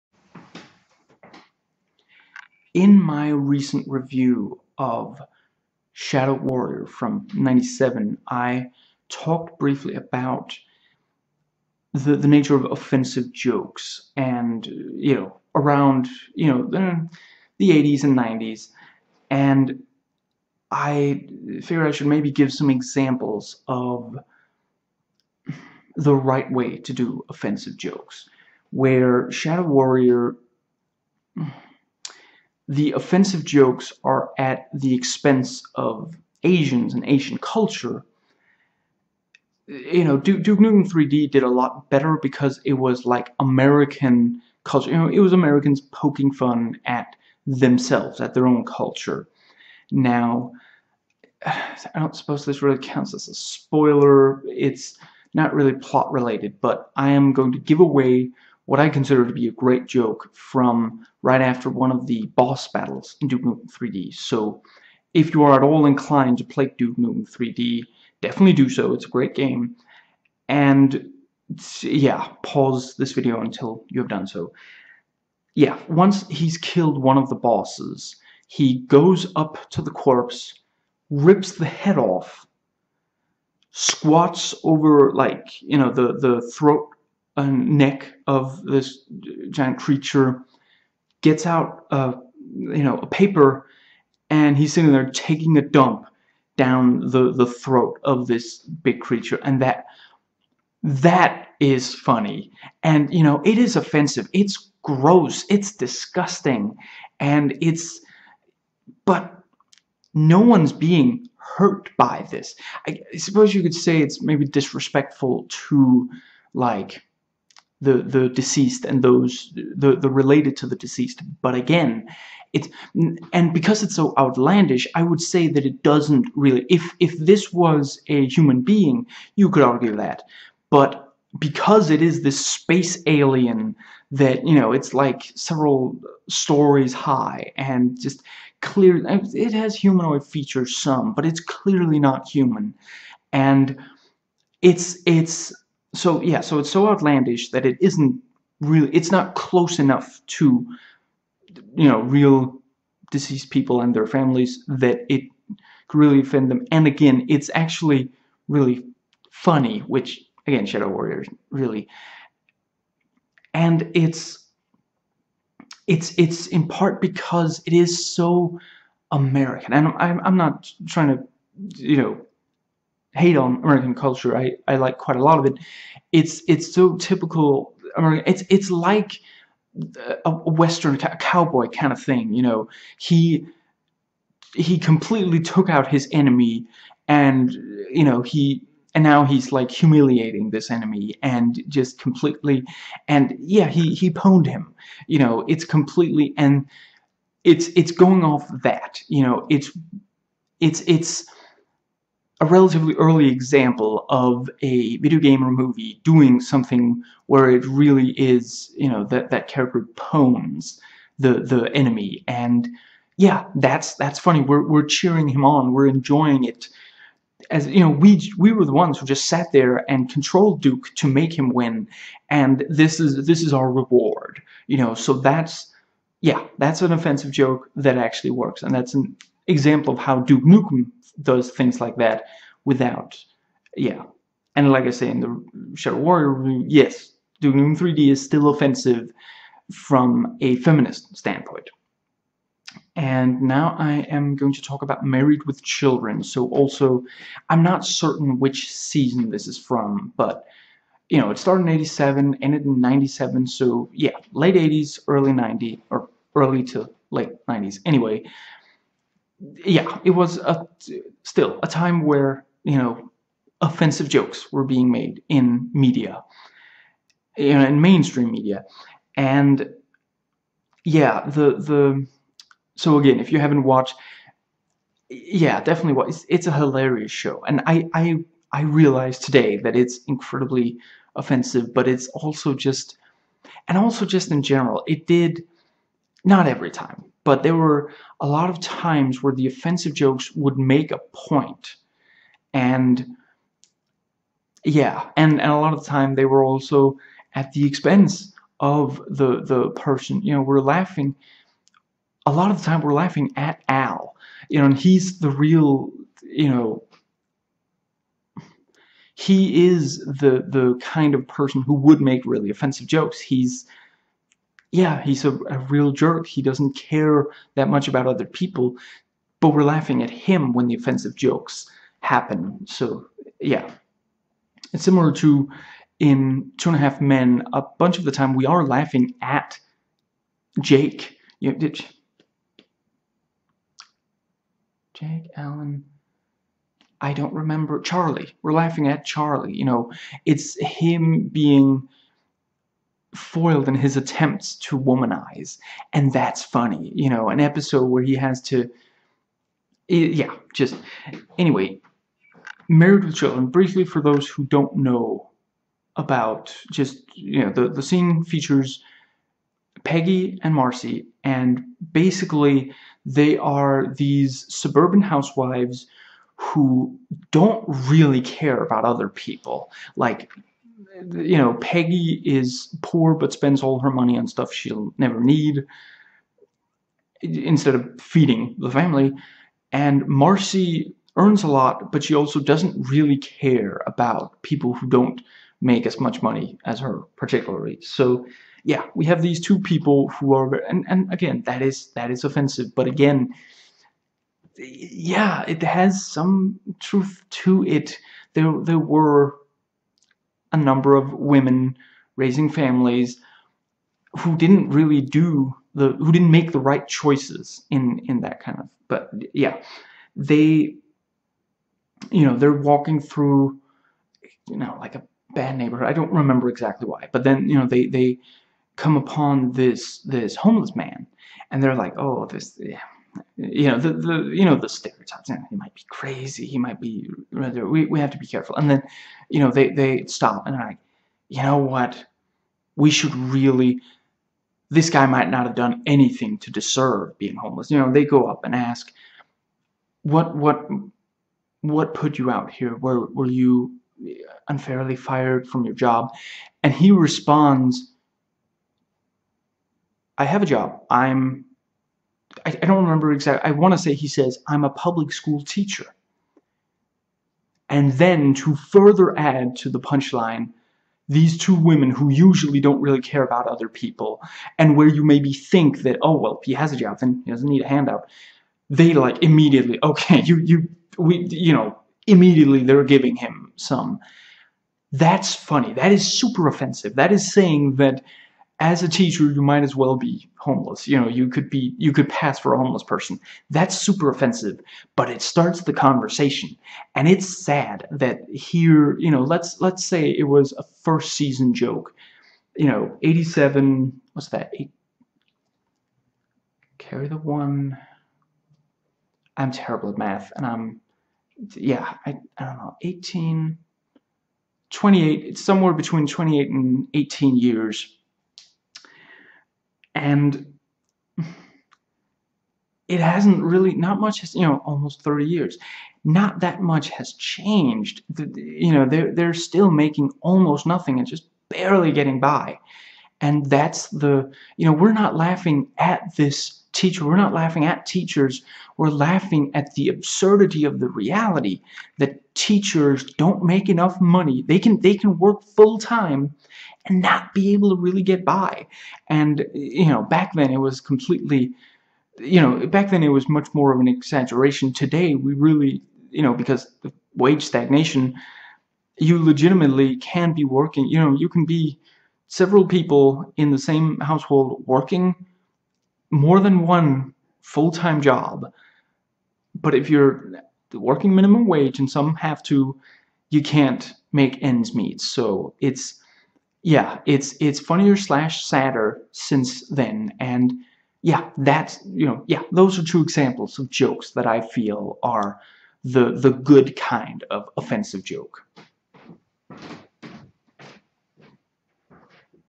The nature of offensive jokes
Timecodes for what I discuss:
00:00 intro
1:15 Duke Nukem 3D joke
4:00 Married With Children: background
6:50 the joke itself